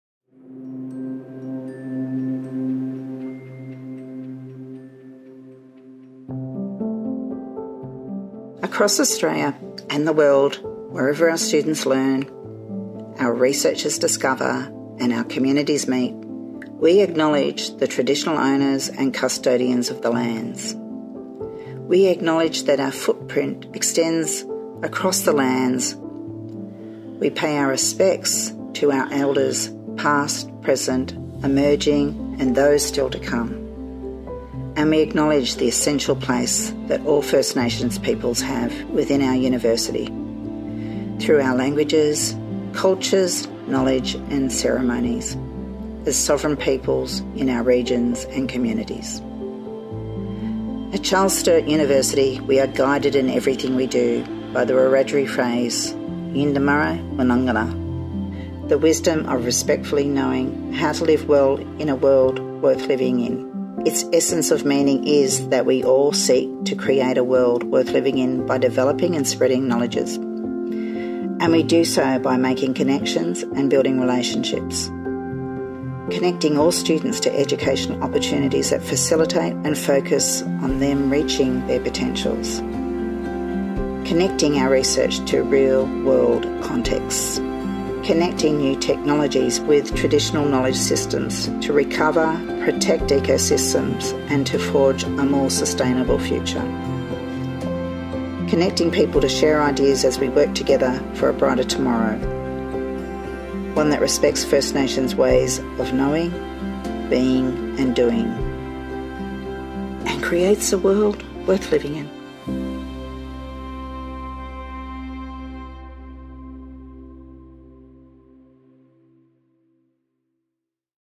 Respecting First Nations ways of knowing, being and doing.
At Charles Sturt, our communities respect First Nations ways of knowing, being and doing. It’s how we work together to connect new technologies with traditional knowledge systems to protect and recover our ecosystems. It’s how we connect all students to educational opportunities to allow them to reach their full potentials. It’s how we create a world worth living in.

As National NAIDOC Week 2021 begins tomorrow, we respectfully acknowledge the ongoing connections to Country, cultures and languages of First Nations communities, students and staff.